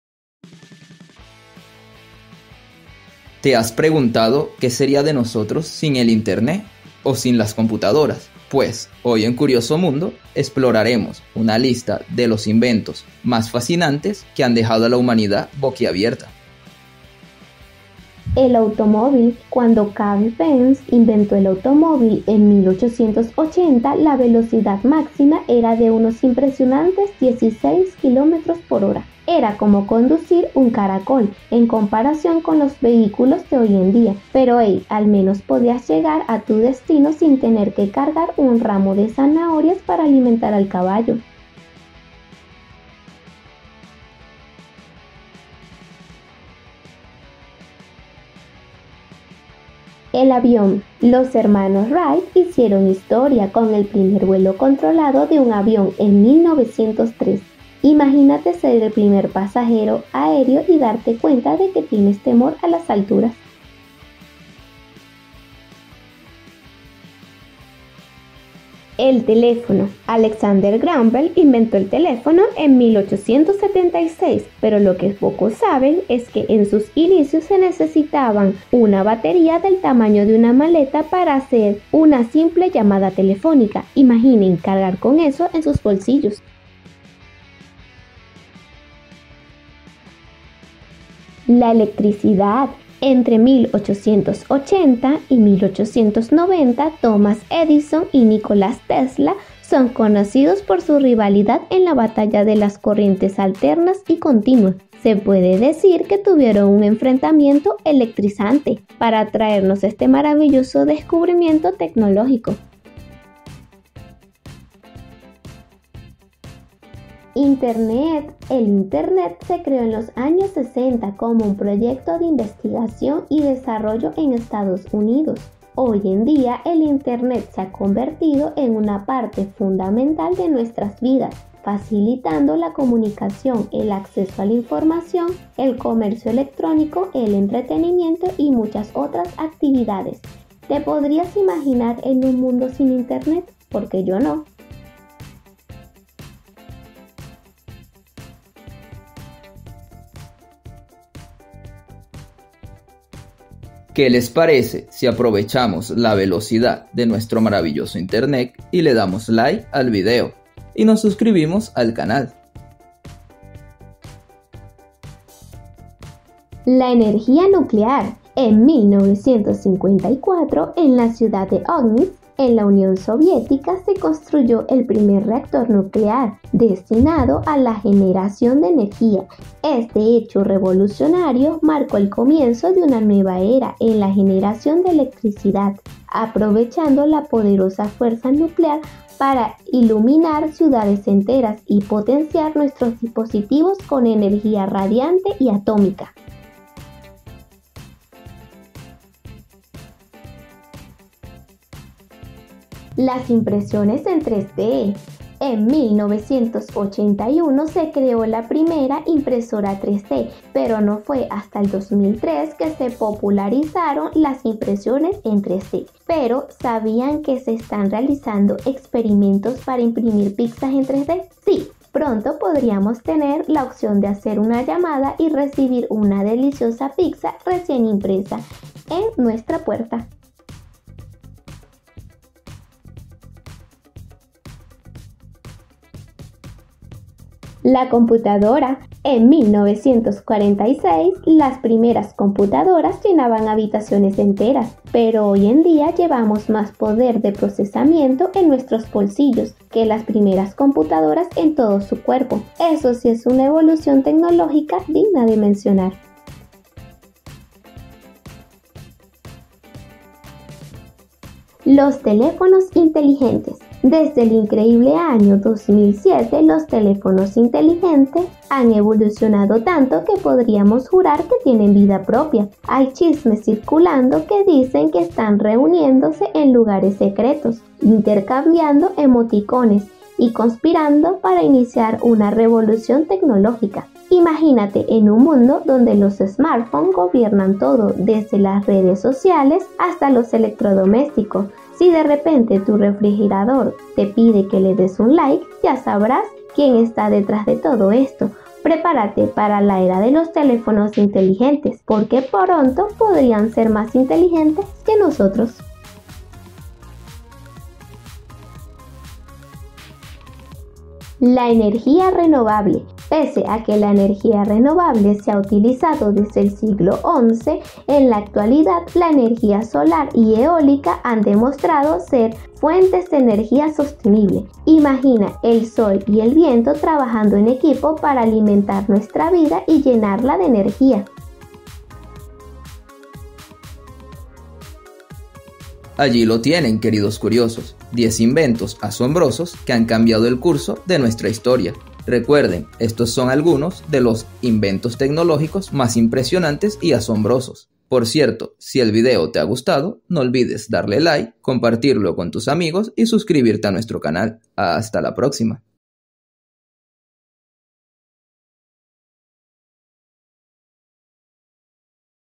10 INVENTOS increíbles que cambiaron la historia
En la actualidad. Cada día hay nuevos inventos que hacen la vida más fácil e interesante. Mucho de ellos han cambiado el rumbo de la historia como la conocemos. Prepáresen para una recopilación de algunos Inventos asombrosos que están a otro nivel y que sin ellos el rumbo de la historia seria diferente.

Hoy en CuriosoMundo hablaremos desde los telefonos, hasta los automoviles.